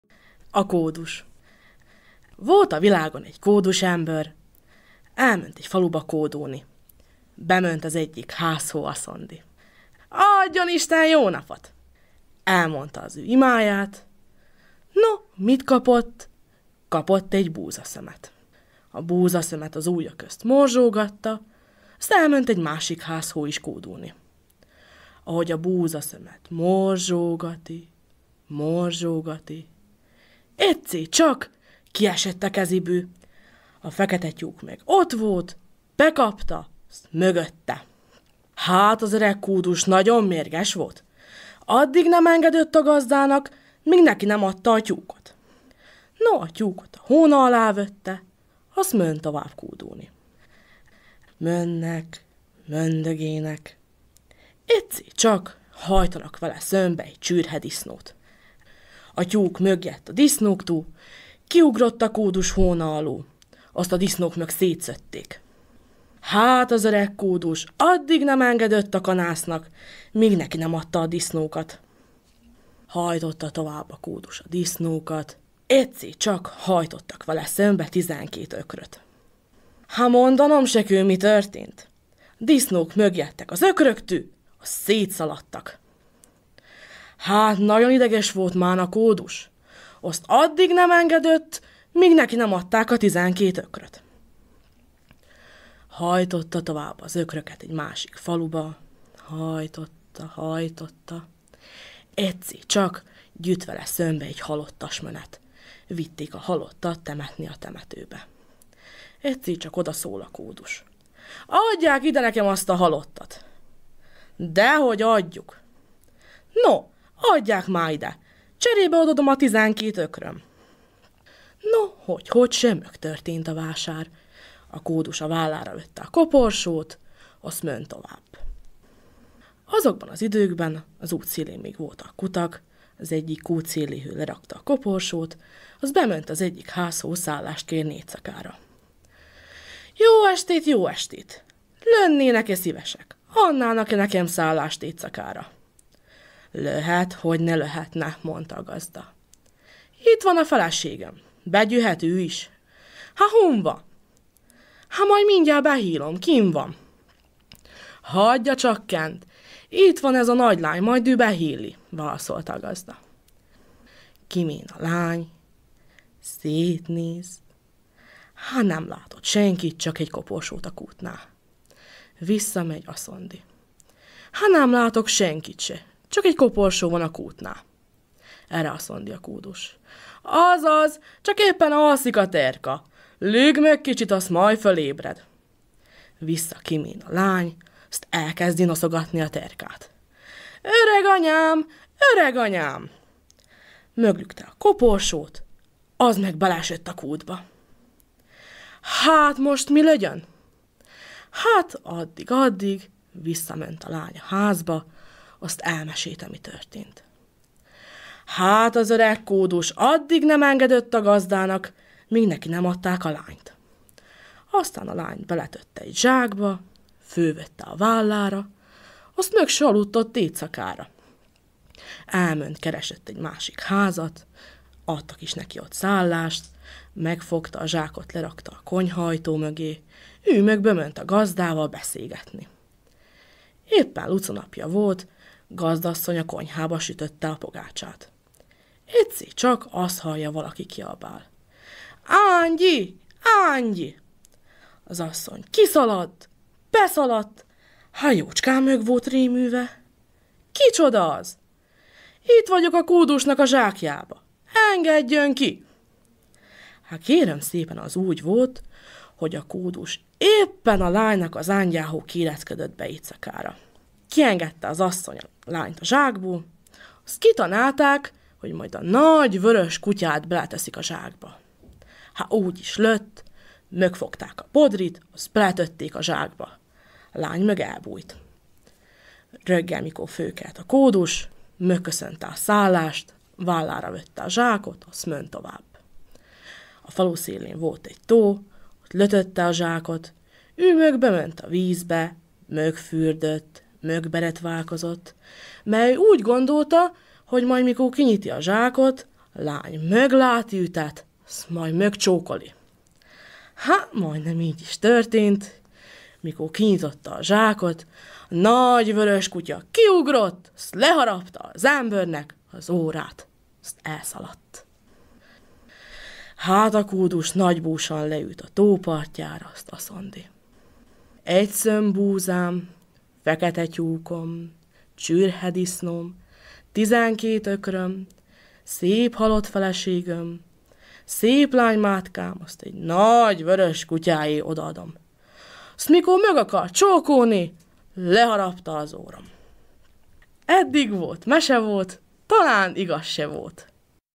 A kódus
A népmesét Nagy Alexandra, Penavin Olga-díjas ludasi mesemondó meséli el.
Alexandra a Ludasi Népmesemondó Műhely tagja.

Felkészítő: Szabó Réka
Hanganyag: Szerbiai Mária Rádió